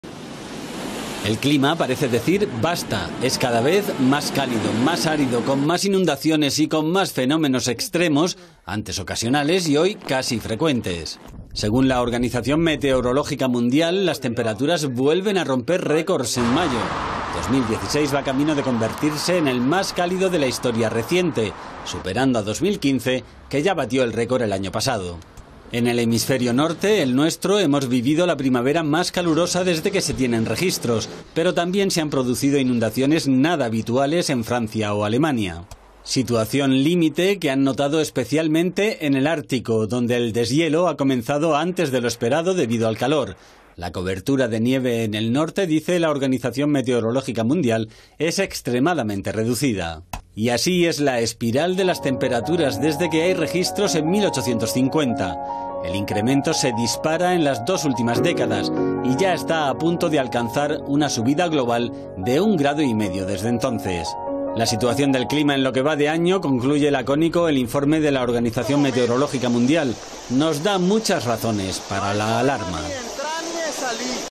La temperatura global batió nuevos récords el pasado mayo
Esta primavera se ha convertido en la más cálida que se ha registrado en el hemisferio norte. Datos que, según la Organización Meteorológica Mundial, ofrecen muchas razones para la alarma.